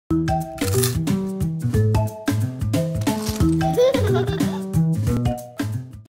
Crackling Crunch of the Childhood | Cornado | You can't
What's the most daring thing you have ever done? Well resisting Cornado definetly won't be one of those! When you give in to your temptations and enjoy finger full of Cornado—the ultimate snacking buddy for the GenNex—you feel like a winner till the very last bite. So, grab a pack of Cornado and enjoy snacks laden with irresistible flavours and goodness of crunchy corn cones! Try once and you won't be able to stop ever again because the taste is so good that you can't DareToResist.